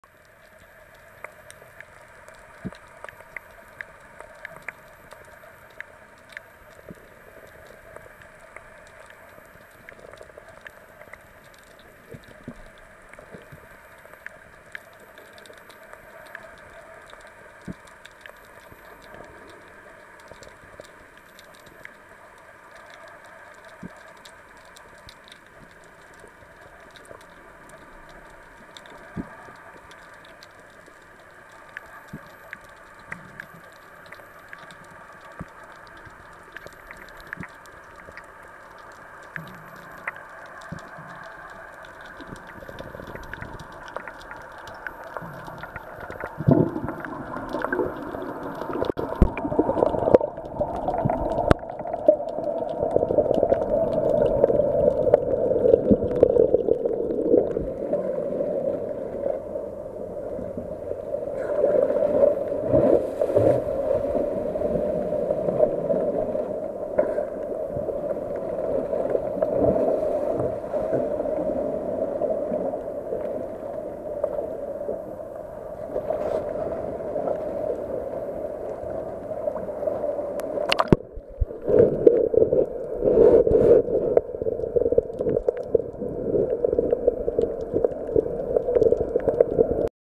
tra Re di Triglie, Castagnole e Spirografi
I colori di una delle fessurazioni nella roccia sotto Capo Zafferano, popolata da Re di Triglie, Castagnole e piccoli Spirografi